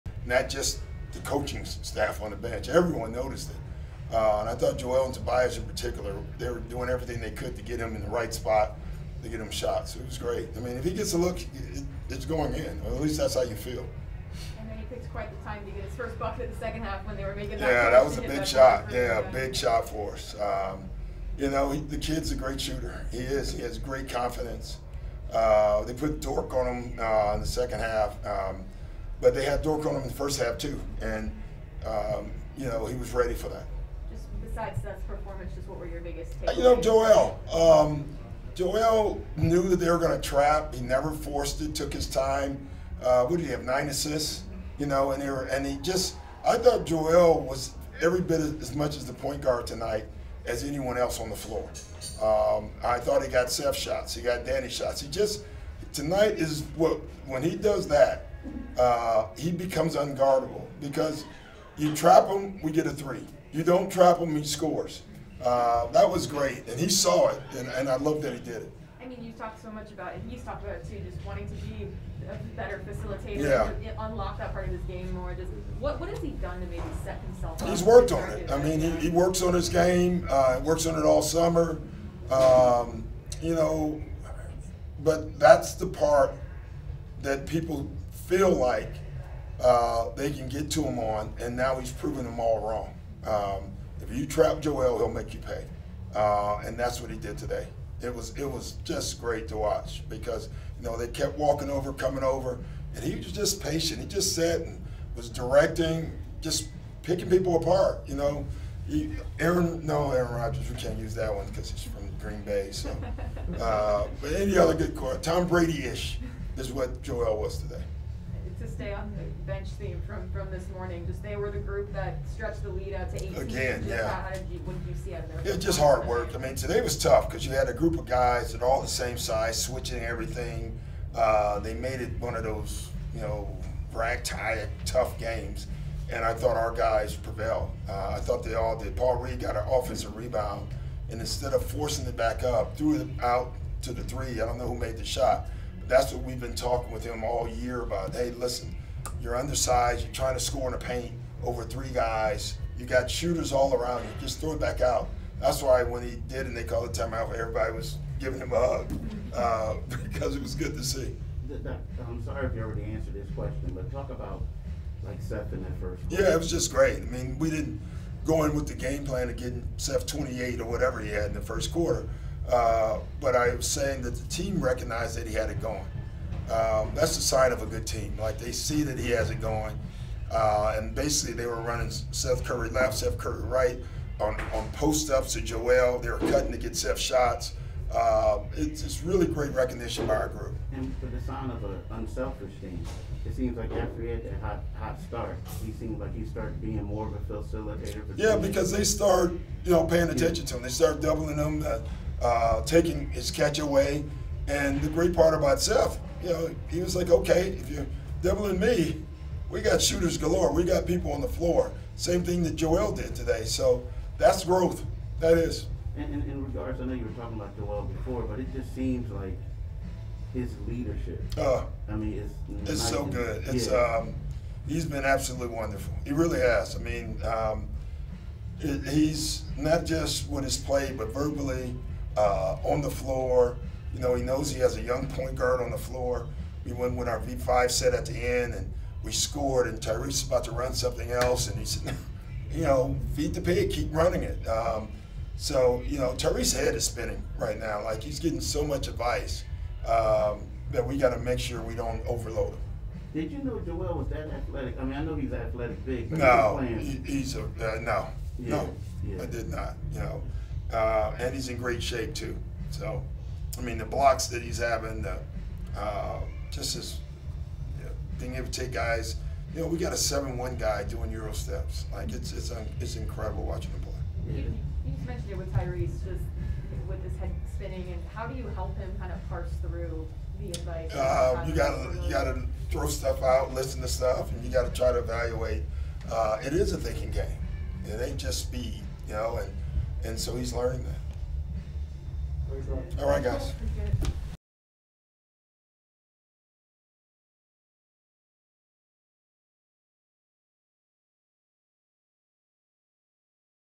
Doc Rivers Talks Sixers Second Win (10.24.21)
The 76ers (2-1) defeated the Oklahoma City Thunder (0-3), 115-103, on the road on Sunday night.

The Sixers finished with a season-high 25 assists on a 41-for-88 (.466) shooting night. Entering the game with the top 3-point percentage (.443) in the NBA thus far, they were 17-for-41 (.415) from beyond the arc against Oklahoma City.